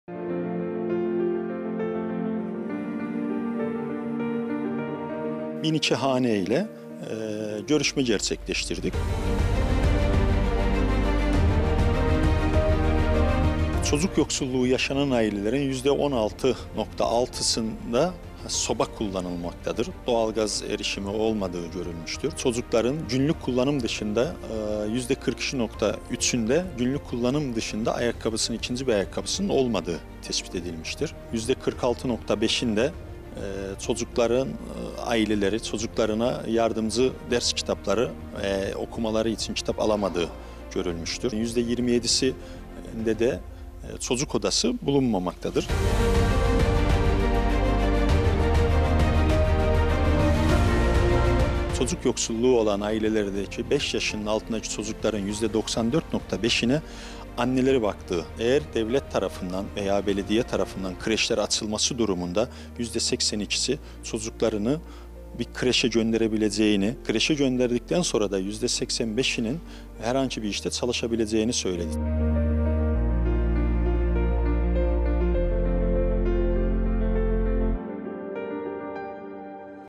İstanbul İstatistik Ofisi’nin Çocuk Yoksulluğu Araştırması
İstanbul İstatistik Ofisi'nin çocuk yoksulluğu araştırmasında çarpıcı sonuçlar ortaya çıktı.

ayrıca İBBTV’yi diğer mecralarda da takip etmek için:
Makaleler, programlar ve canlı yayın: